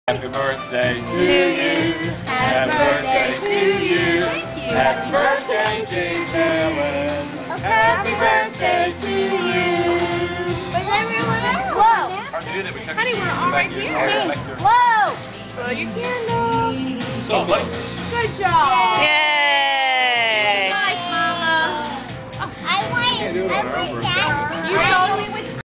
Happy Birthday James!
We were at Chuck E. Cheese celebrating James' birthday!